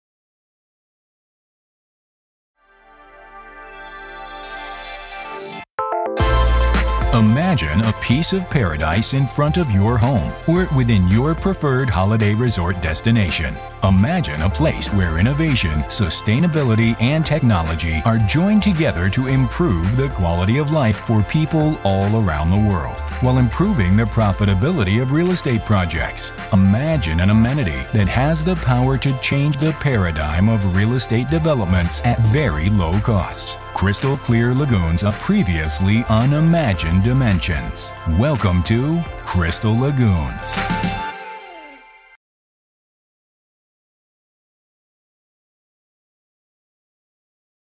Introducing The Lagoon at Windsong Ranch
We are extremely excited to announce our newest Amenity Center, The Lagoon at Windsong Ranch! The centerpiece of this inspired facility will be a 5-acre crystal clear lagoon by the world-renowned amenity company Crystal Lagoons®. Discover more at windsongranchliving.com